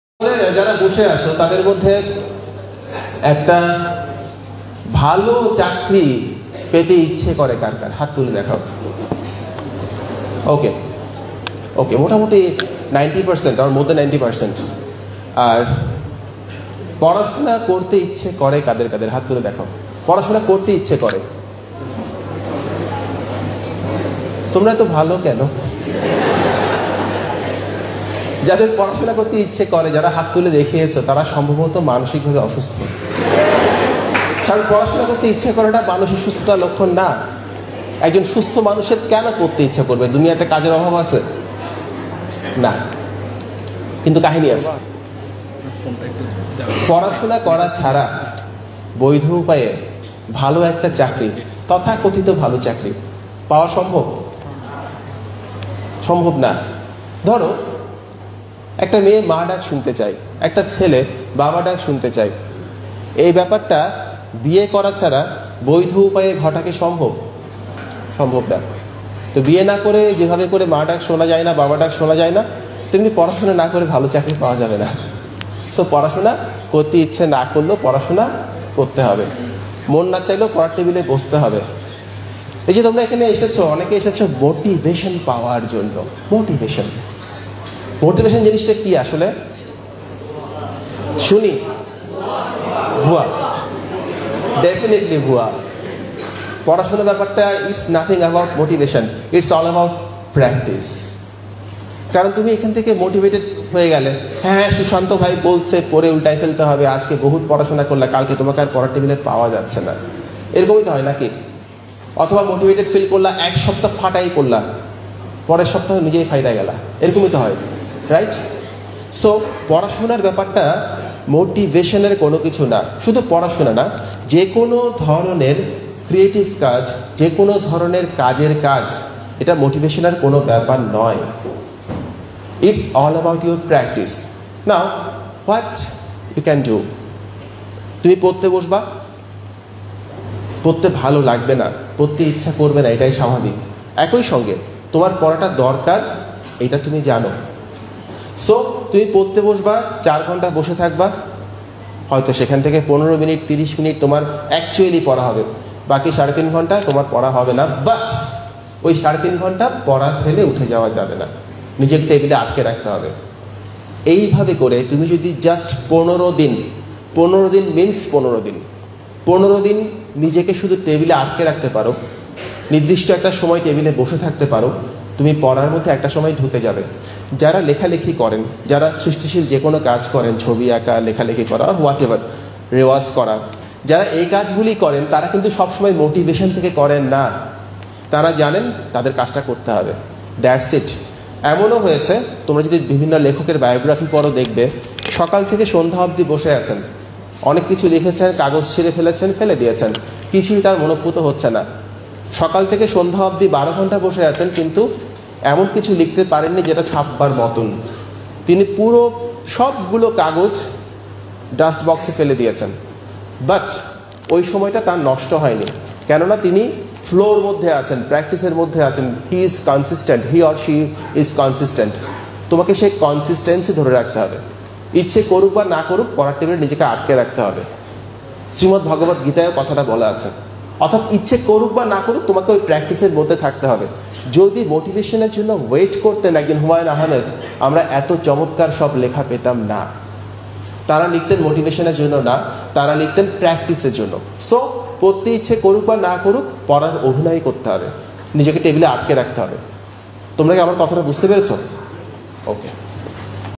চাকরি পেতে ইচ্ছে করে | পড়তে ইচ্ছে করে না | Motivational Speech | Inspirational Video | Sushanta Paul
Keynote Speaker
Sushanta Paul
 First-Class Gazetted
Government Officer,
Writer, Influencer

সুশান্ত পাল
৩০তম বিসিএস এ সম্মিলিত মেধা তালিকায় (প্রথম)

      সাজ্জাদ হোসেন সুমন